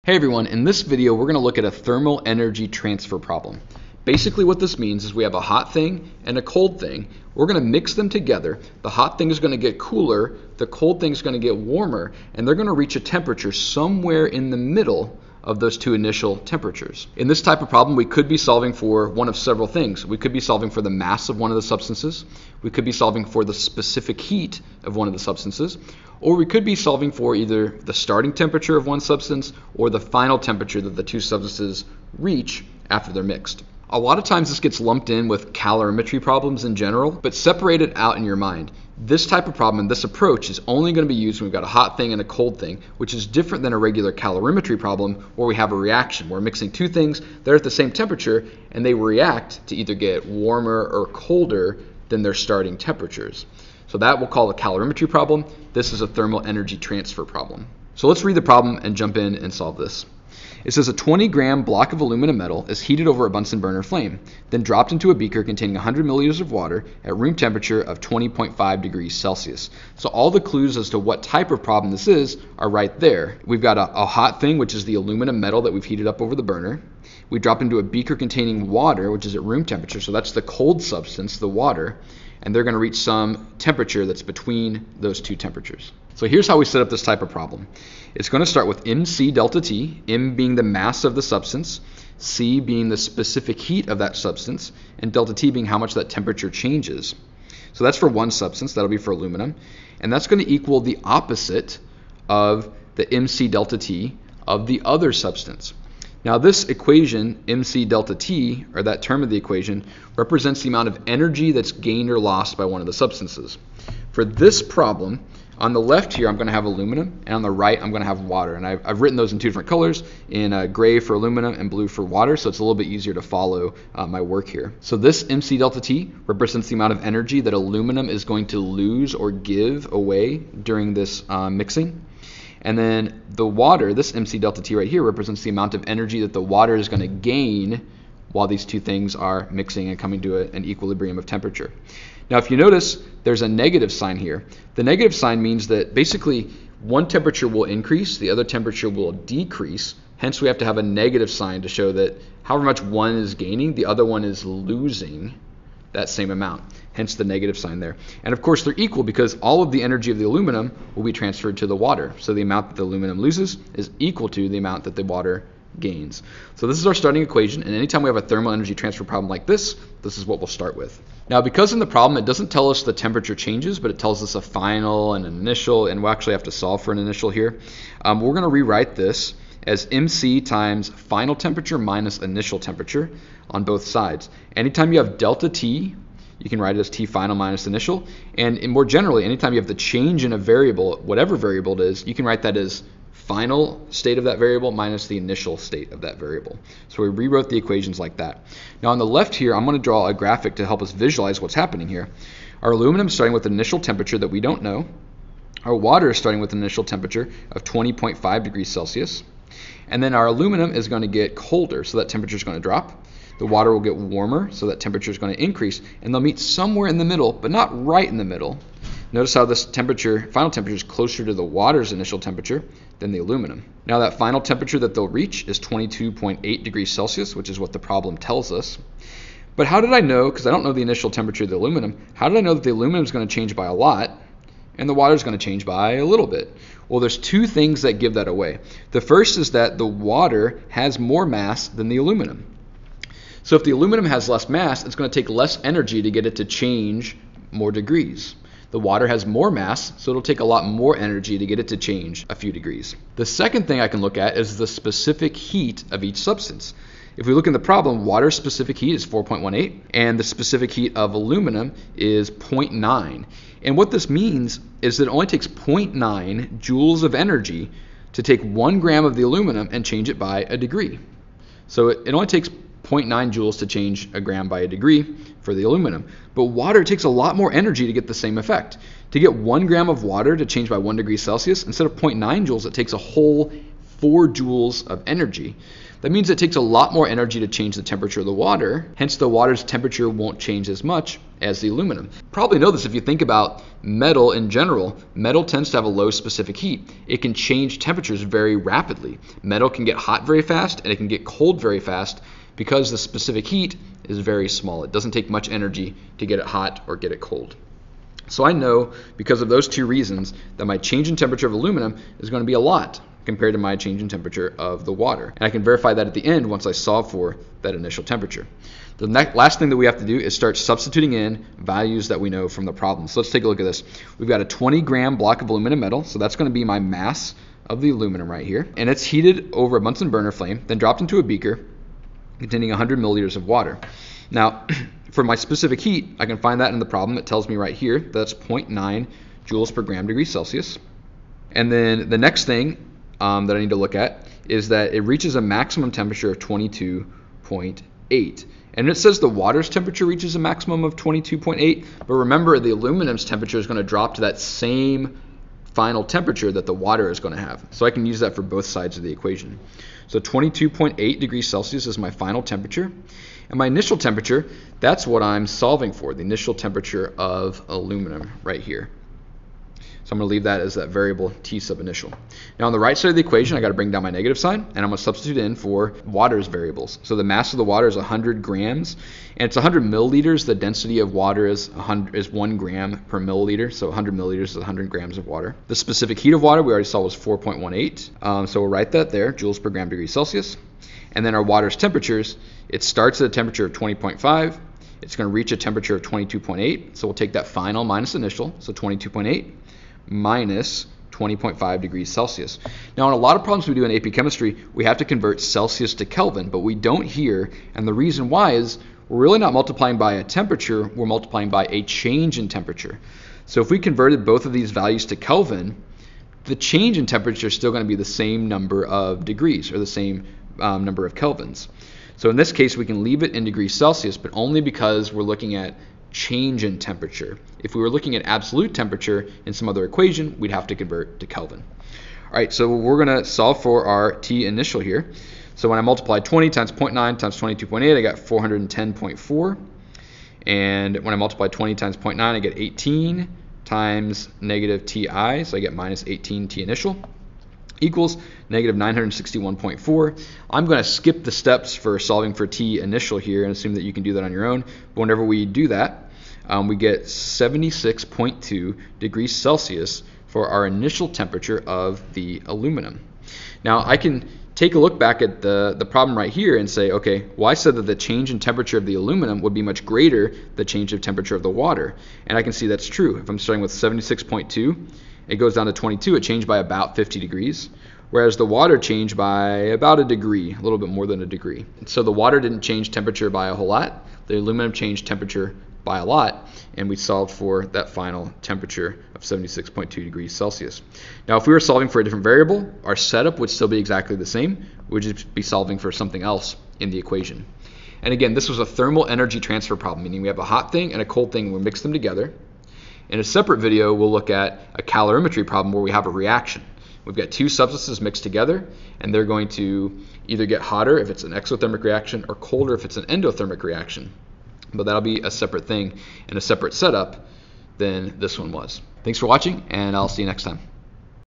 Thermal Energy Transfer Problem | How to Solve in AP Chemistry
In this video, I show how to solve a thermal energy transfer problem (not a regular calorimetry problem!) in AP Chemistry. I also share some qualitative reasoning about which substance will have the greatest change in temperature, based on the mass and specific heats of each substance. (The AP Exam loves to test on qualitative reasoning like that!)